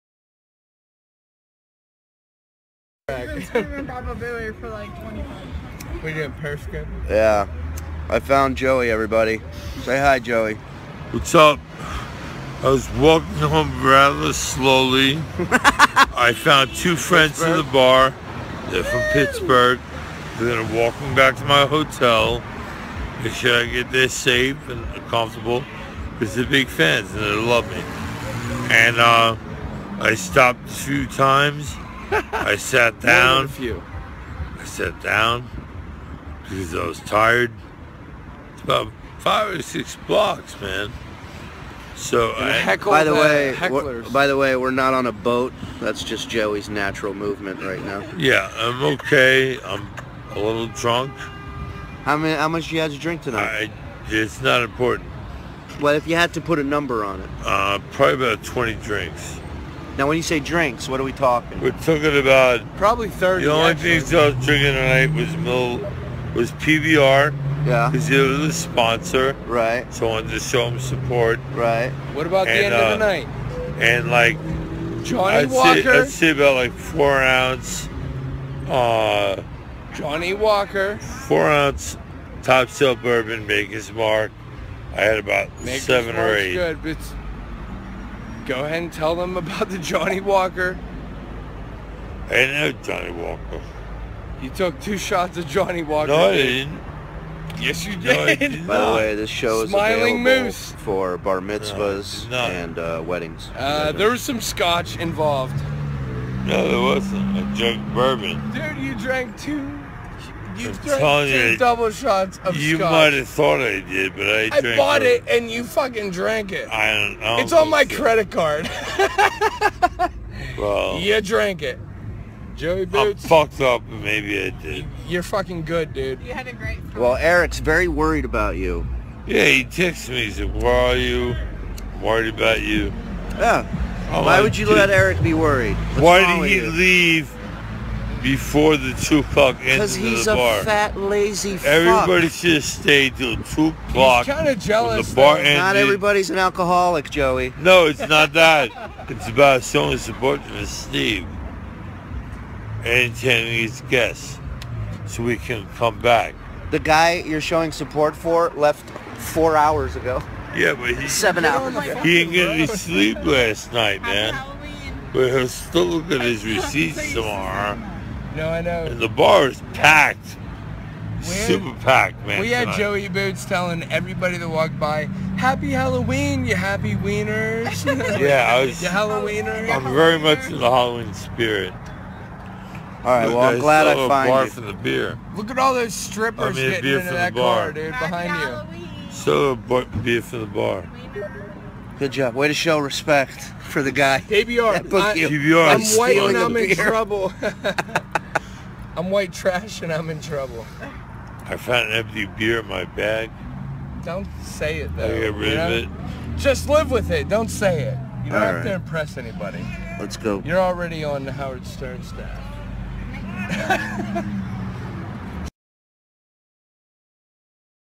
Joey Boots wasted out of his mind on Halloween
Shuli booked Joey and High Pitch, and look what happens. I feel sorry for Shuli .. its suppose to be a professional gig.